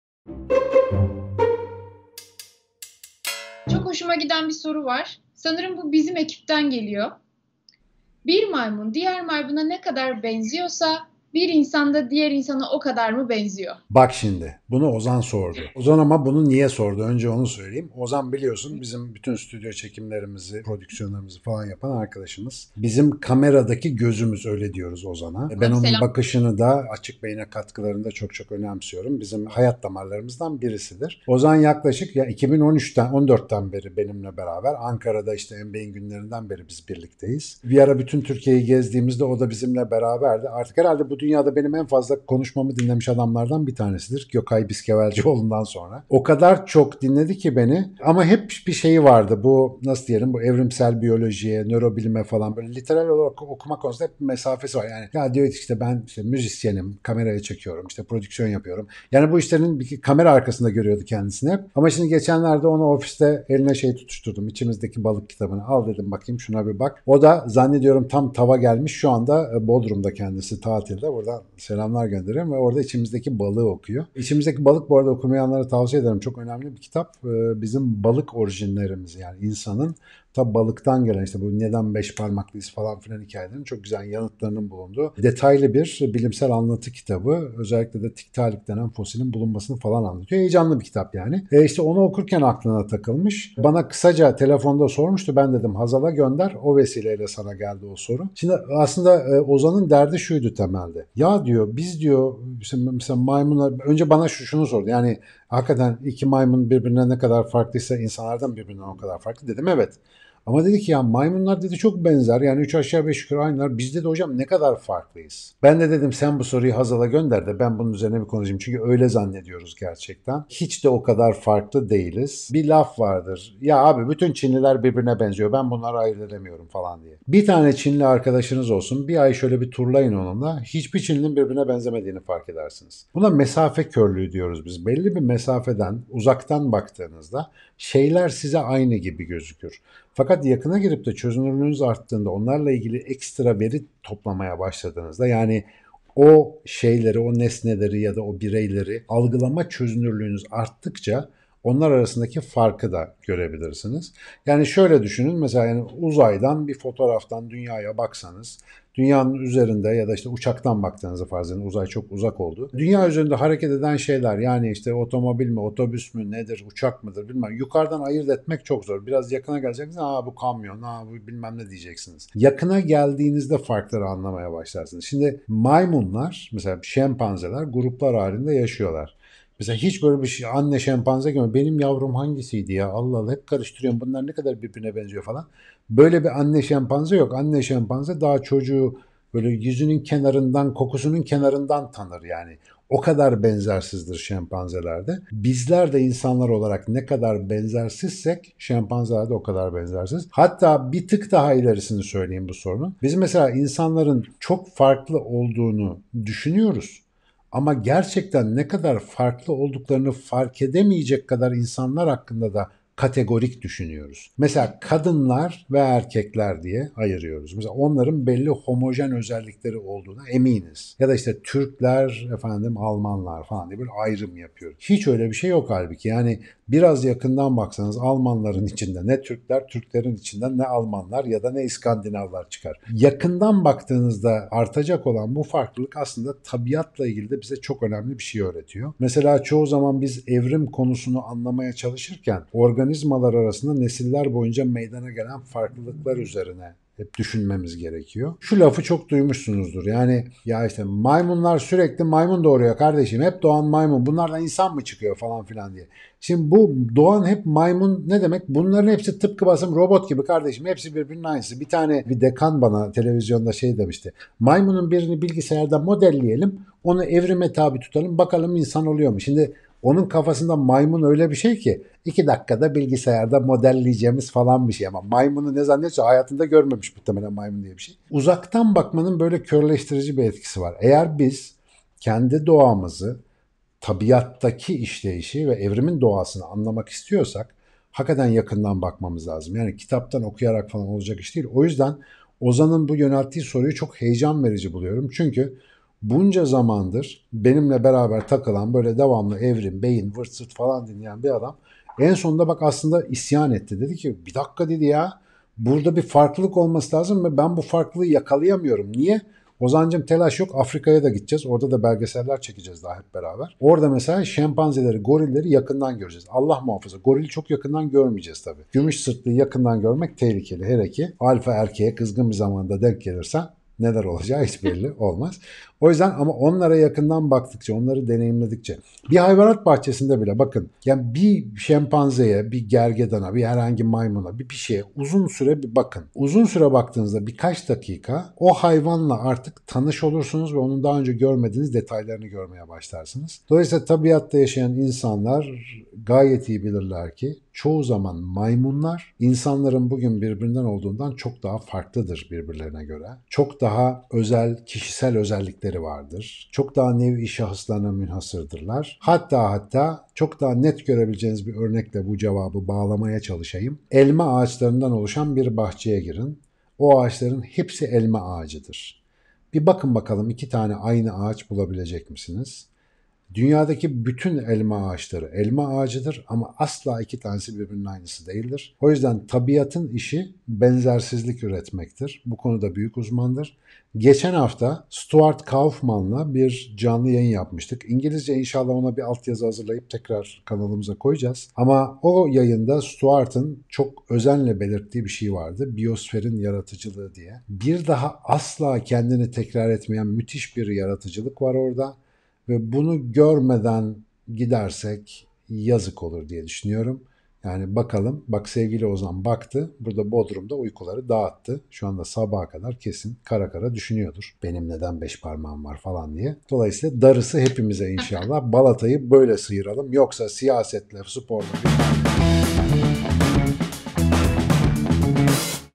SoruYorum - İki Maymun Birbirine Ne Kadar Benziyorsa, İki İnsan Da Birbirine O Kadar Mı Benziyor ?
SoruYorum programımızdan herkese merhaba. İki maymun birbirine benziyorsa, iki insan da birbirine o kadar mı benzer? Aslında hiçte o kadar farklı değiliz. ''İki Çinli birbirine benziyor ben onları ayırt edemiyorum'' deriz. Ama bir Çinli arkadaşımız olsun, bir ay birlikte vakit geçirin. Hiç bir Çinli birbirine benzemez. Sebebi ve sorunu cevabı videonun devamında.. İyi seyirler.

SoruYorum - İki Maymun Birbirine Ne Kadar Benziyorsa, İki İnsan Da Birbirine O Kadar Mı Benziyor ? videomuzu beğendiyseniz diğer videolarımıza göz atmayı unutmayın;